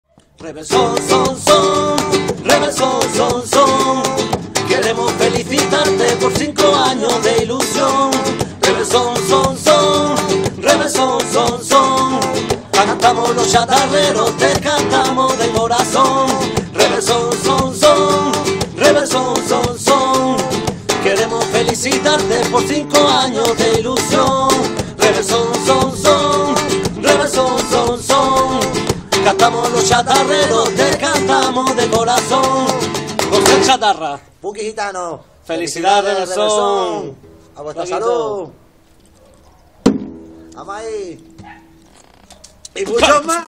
48. 5 Años Rebel Sounds :: Jose "El chatarra" (Catalunya)
Video completo del saludo y felicitación que hace Jose "El chatarra" a toda la comunidad sonora Rebel Sounds